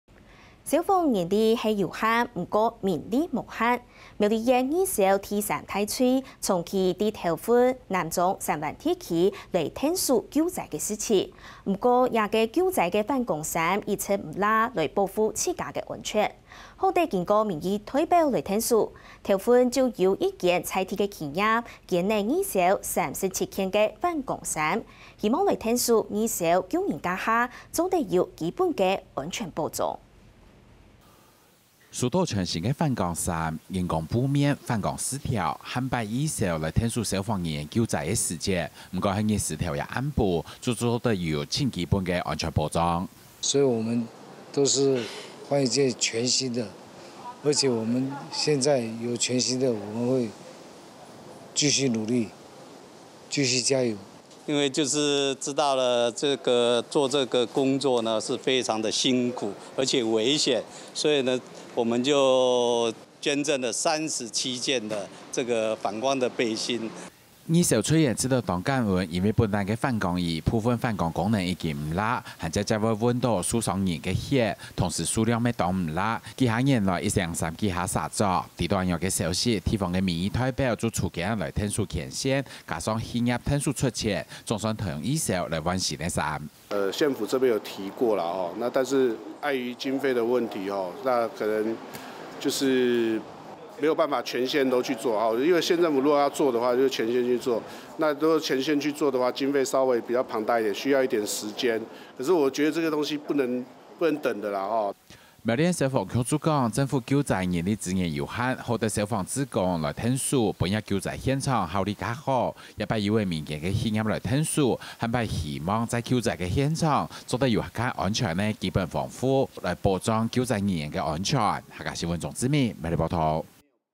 苗栗義消反光衣數量不足 企業捐衣助救災【客家新聞20201026】
消防人力有限，但民力無限，苗栗縣義消第三大隊長期在頭份、南庄、三灣地區協助救災事宜，卻苦無足夠的救災反光衣，保護自身安全，所幸經過民意代表牽線，頭份有一家在地企業捐贈義消37件反光衣，希望協助義消救人時能有基本的安全保障。